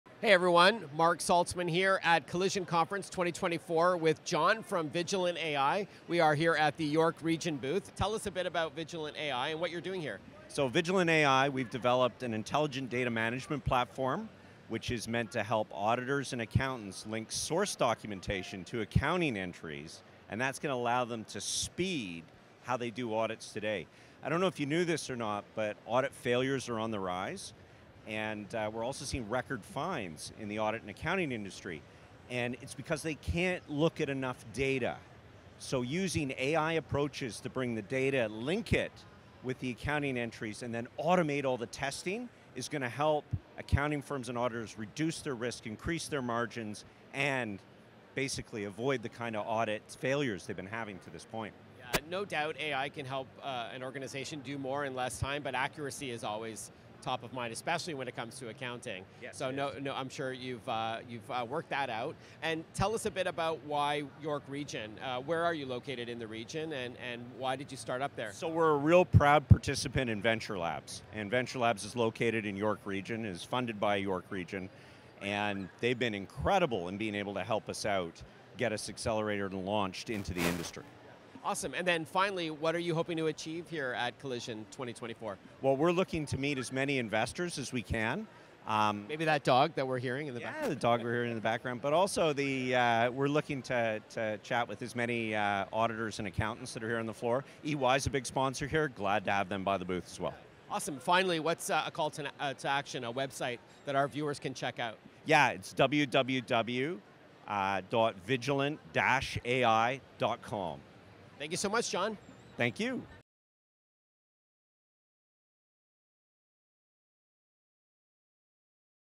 How can data and AI reduce audit risks in the audit process? | Collision '24
John Craig, CEO of Vigilant AI, speaks with Marc Saltzman at Collision Conference in the YRtech media booth about how his company is helping companies reduce risk when it comes to the auditing process by using AI to leverage increased amounts of data and source materials.